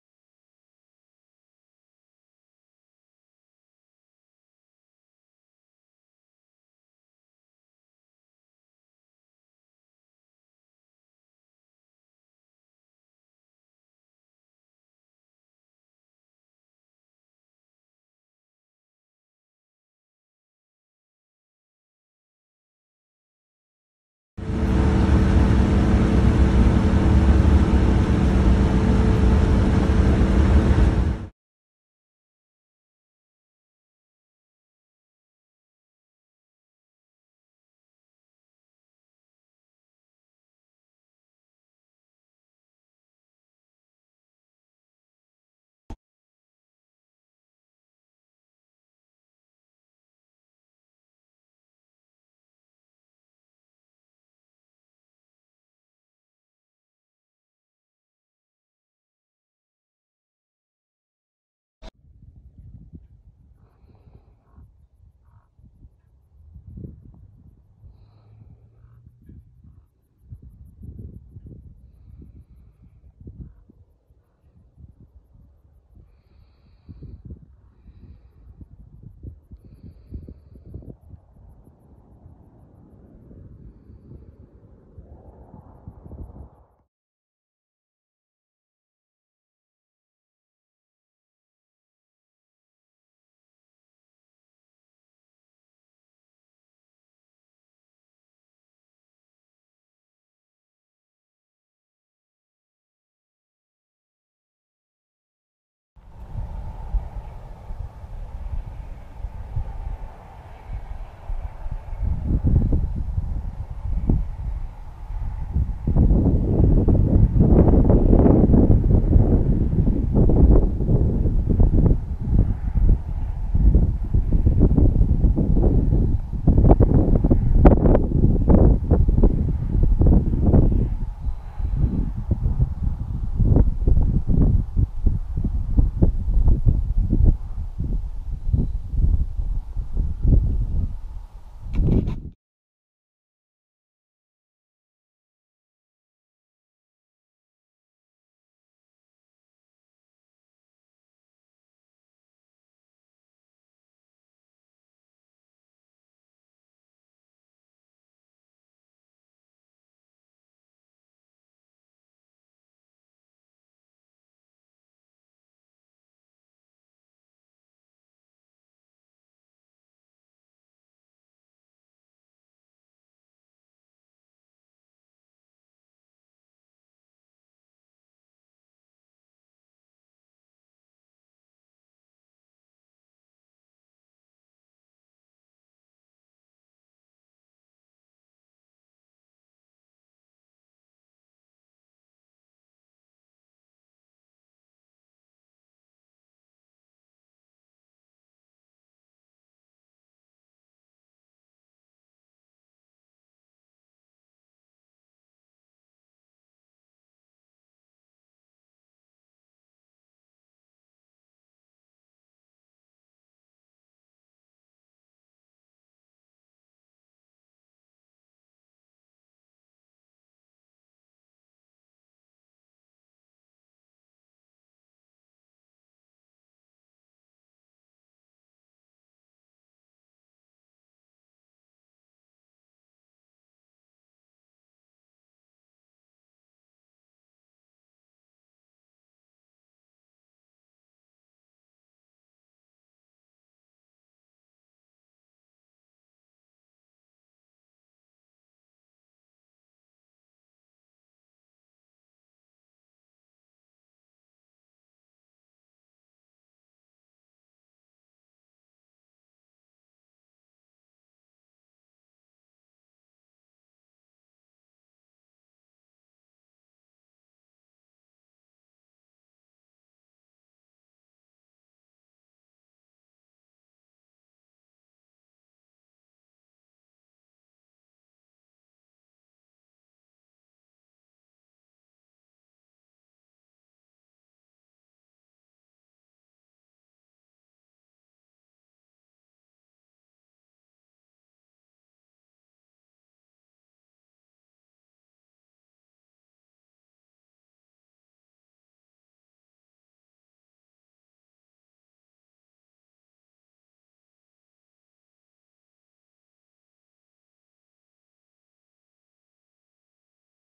DJI Drone Mavic Pro Platinum - Flying Above in The Desert
Flying out in the Desert today and Back home